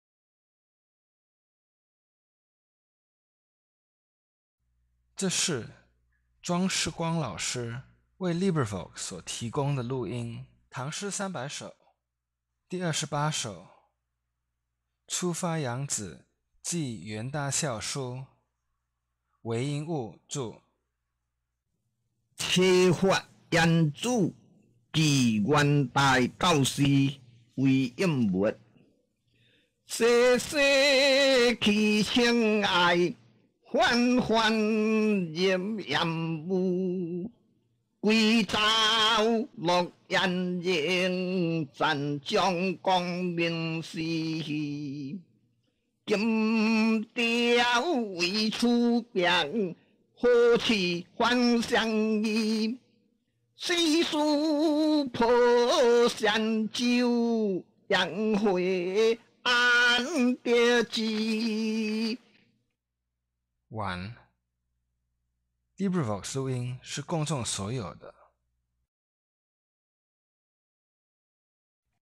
028. Setting Sail on the Yangzi to Secretary Yuan by Wei Yingwu 韋應物 (Hokkien)
Original title: 初發揚子寄元大校書 (Setting Sail on the Yangzi to Secretary Yuan) by Wei Yingwu 韋應物 (Hokkien)